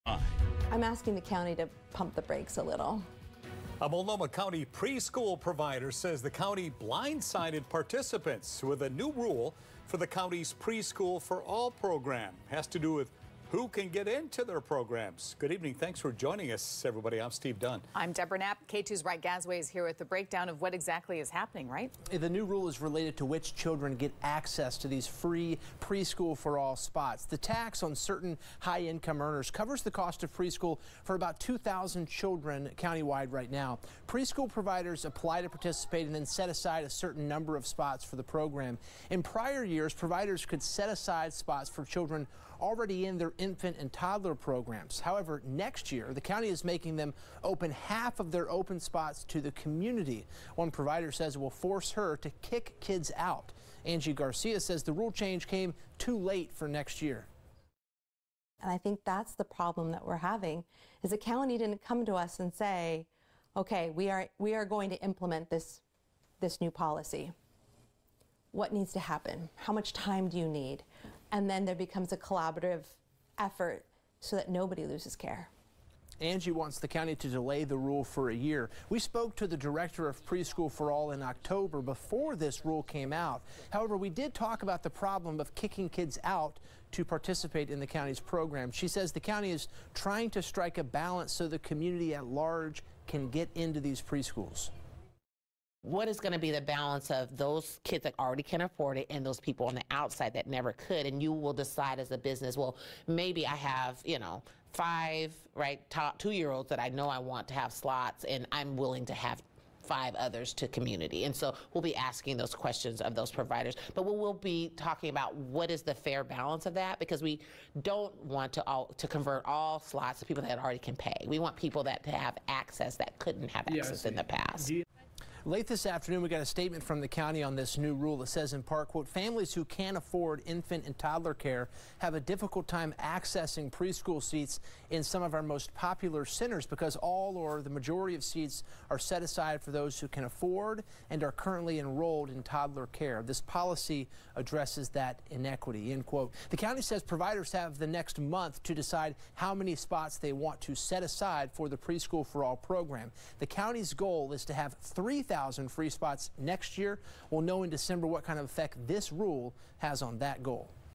Mult. Co. preschool provider says new county rule forces her to kick kids out of her care
A Multnomah County preschool provider said the county blindsided participants with a new rule this week for its Preschool for All program -- tied to who can get into their programs.

Preschool For All -- funded by a tax on certain, high-income earners -- covers the cost of preschool for about 2,000 children right now. Providers apply to participate and then set aside a certain number of spots for the program.

In prior years, providers could set aside Preschool for All spots for children already in their infant and toddler programs -- called continuity of care. However, next year, the county is making them open half of their open spots to the community and only allowing them to set aside 50% of their seats for children already in their care.